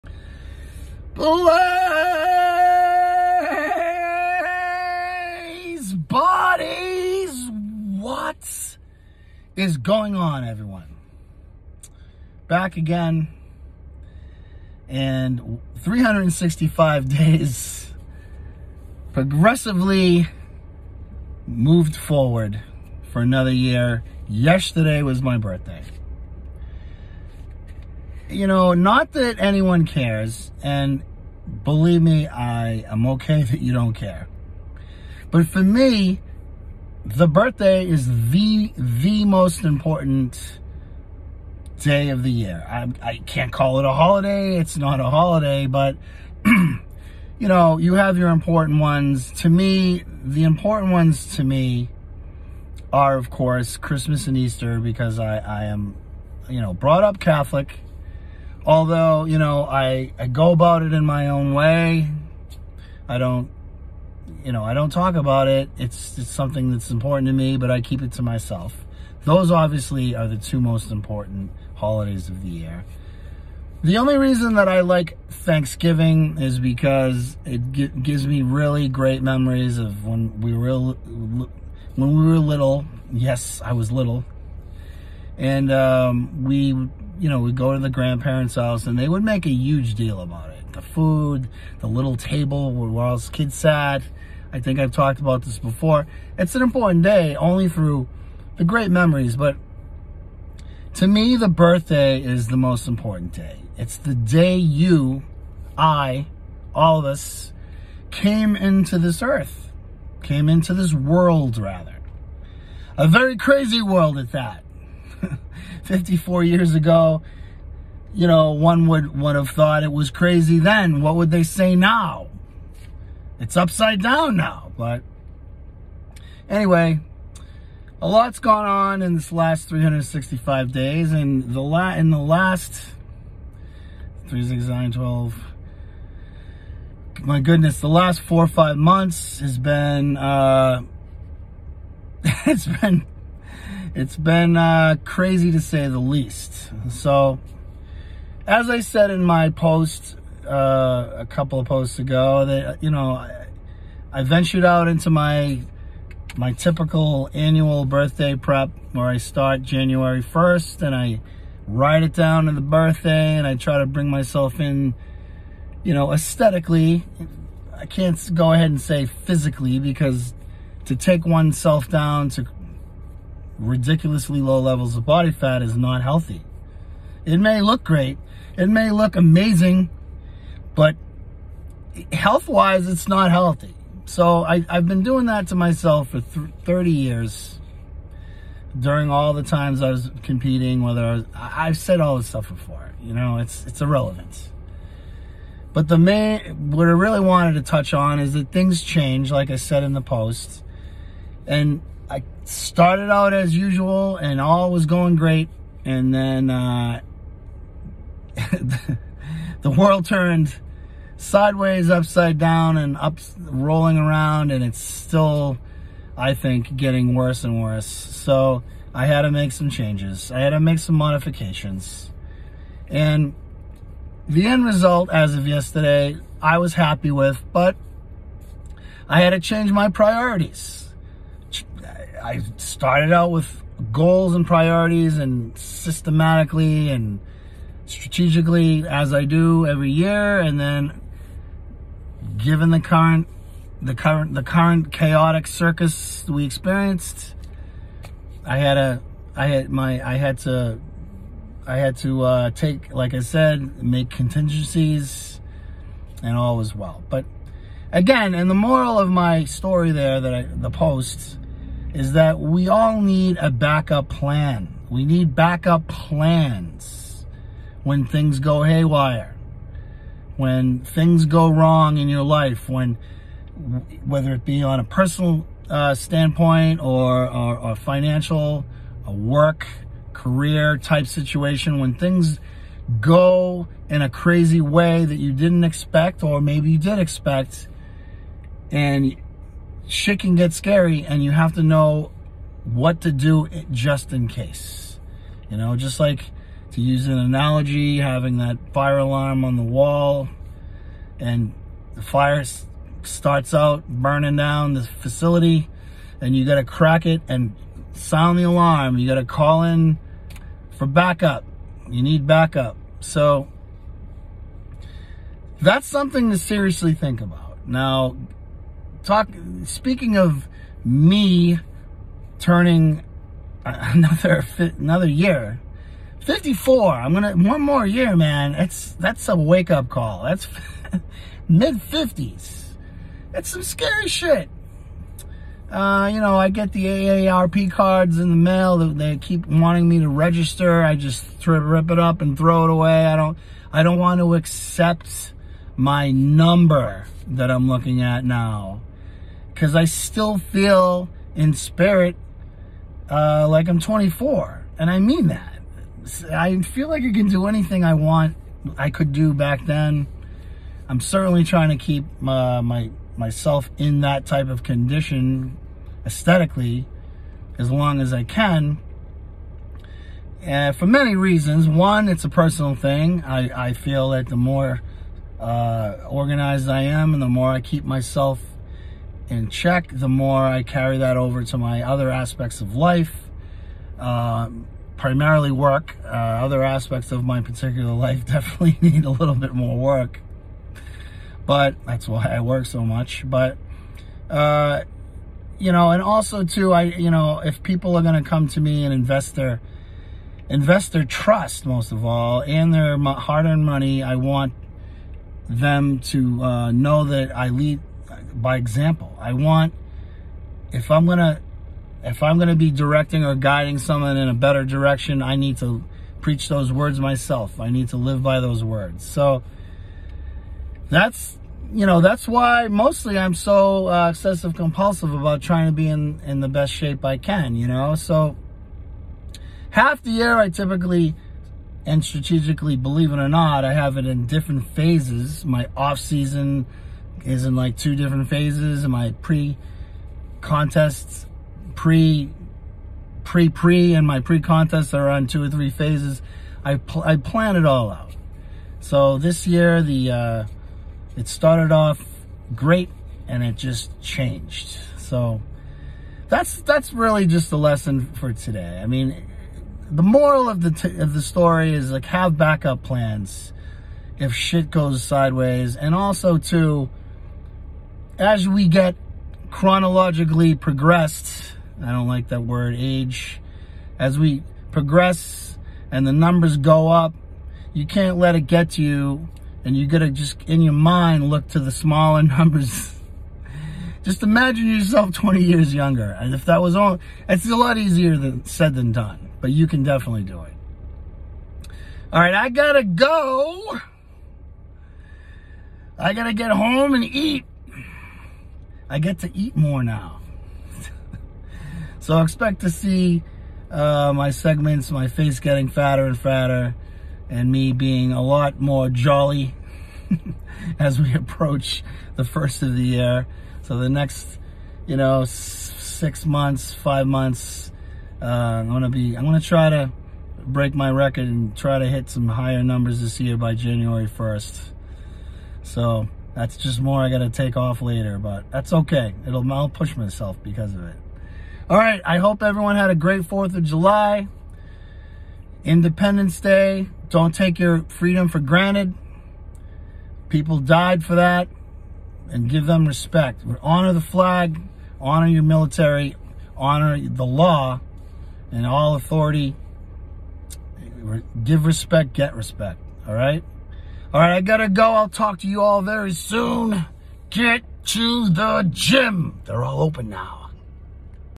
Birthday BackUp
Discussing the importance of ALWAYS having a backup plan in case Life goes sideways